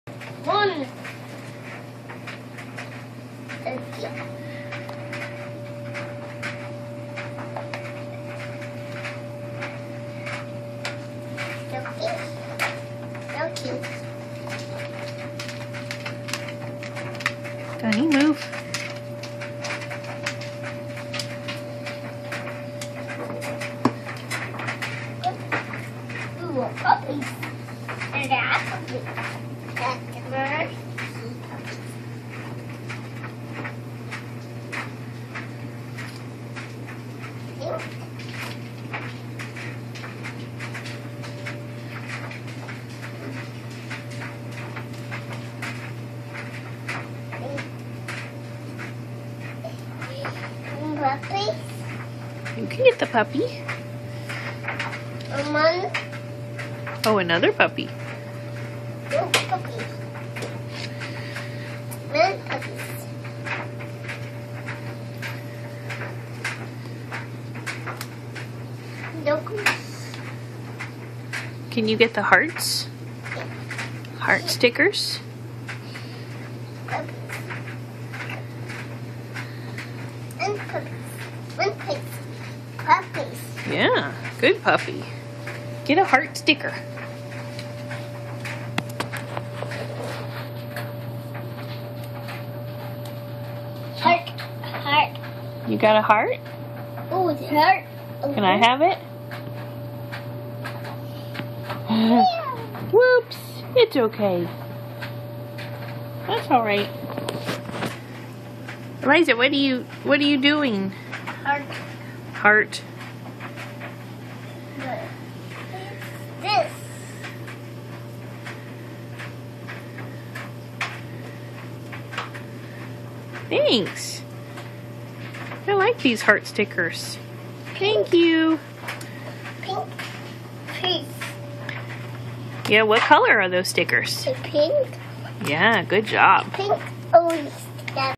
Stickers: Eliza's new favorite thing
Stickers are fun! Excellent for developing the fine motor skills.